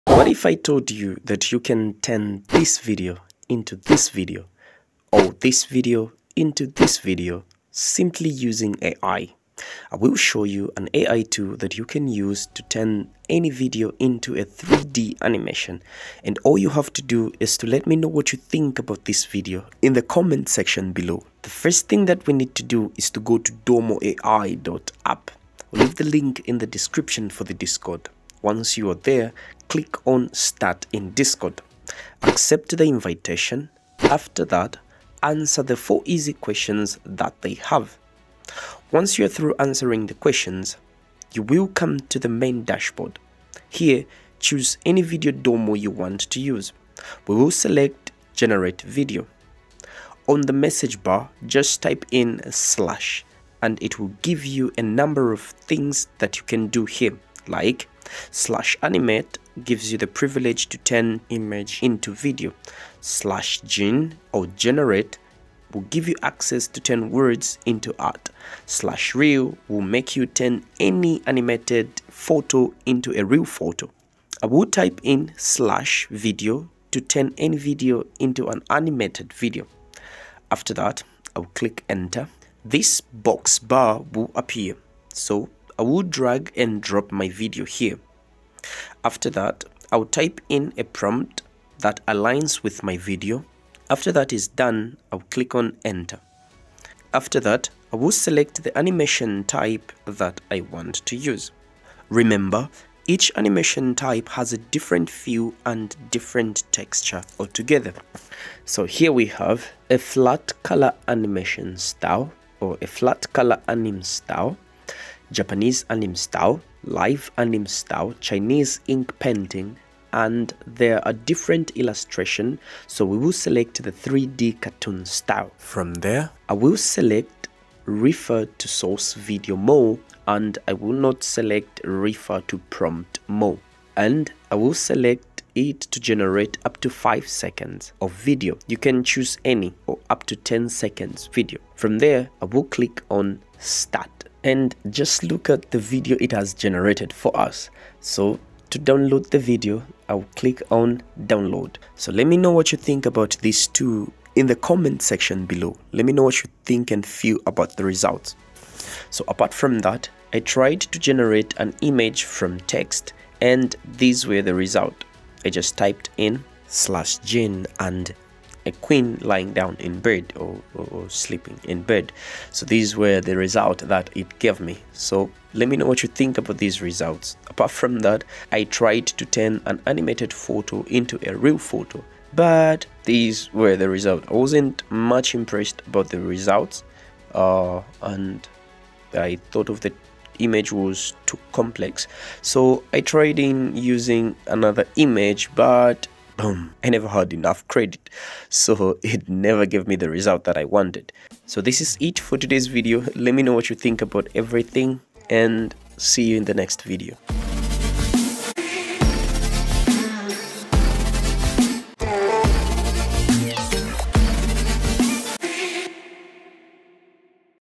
HOW CHANGE ANY VIDEO TO 3D ANIMATION | DOMOAI FULL TUTORIAL
Introducing DomoAI, a groundbreaking application designed to transform any ordinary video into a captivating 3D animated masterpiece. With its advanced artificial intelligence algorithms, DomoAI seamlessly analyzes and enhances your footage, adding dynamic depth and lifelike animations. Elevate your content creation to new heights as DomoAI effortlessly brings your videos to life, offering a user-friendly interface and a wide range of customization options. Unlock the potential of 3D animation with DomoAI, revolutionizing the way you visualize and share your stories. Embrace the future of video editing and experience the magic of dimensional storytelling at your fingertips.